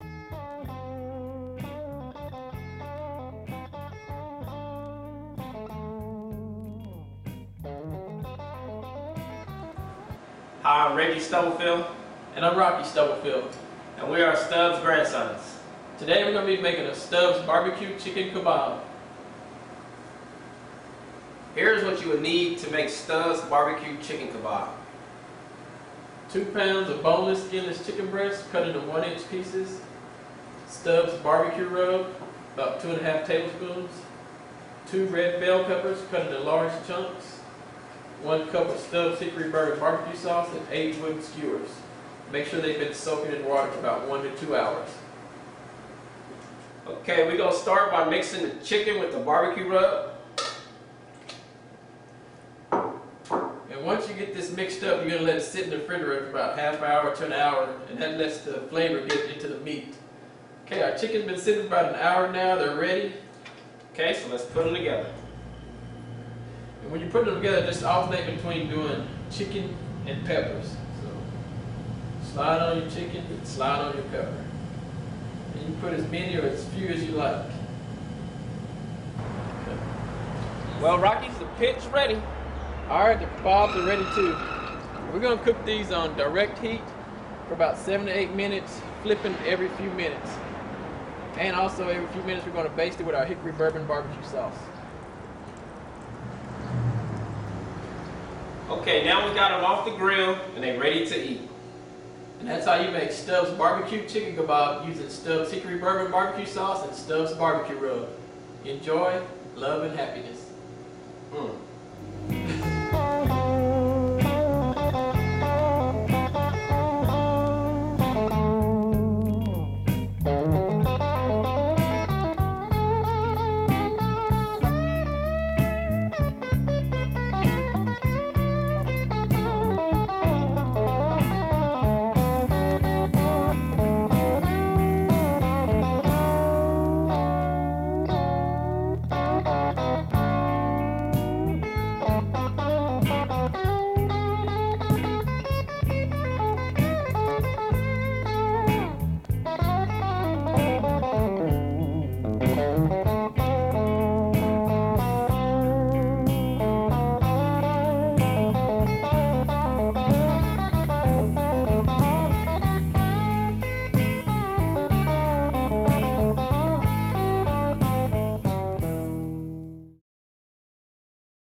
STUBB's BBQ Chicken Kabobs
Watch Rocky & Reggie Stubblefield prepare Stubb's BBQ Chicken Kabobs using Stubb's Hickory Bourbon Bar-B-Q Sauce and Stubb's Bar-B-Q Spice Rub.